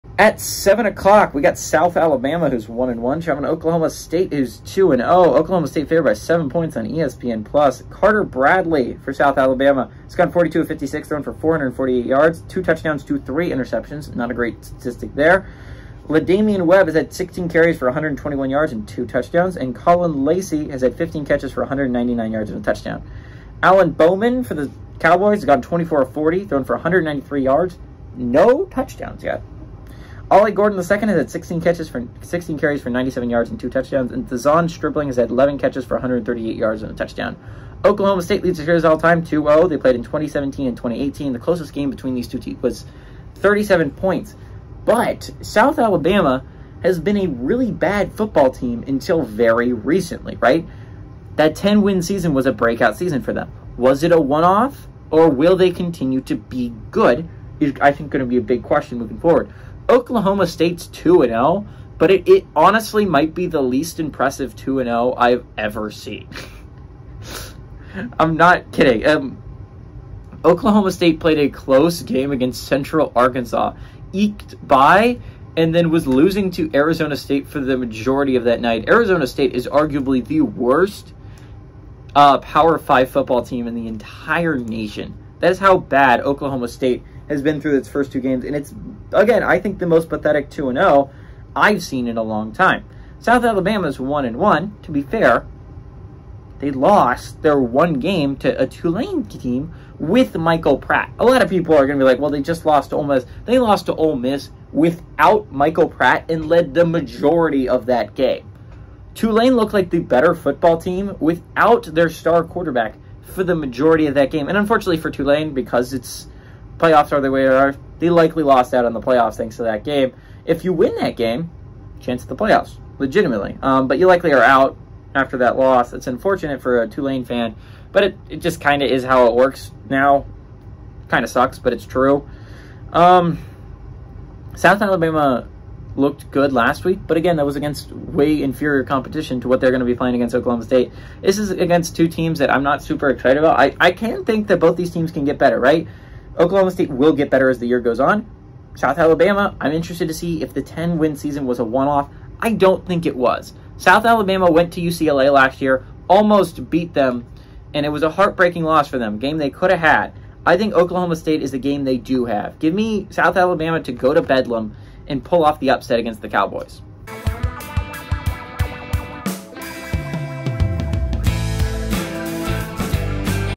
South Alabama @ Oklahoma State
In this video, GGB predicts one of the biggest games of the week. Will the Jaguars pull off the upset on the road or will the Cowboys stay undefeated? The only way to know is to watch the video.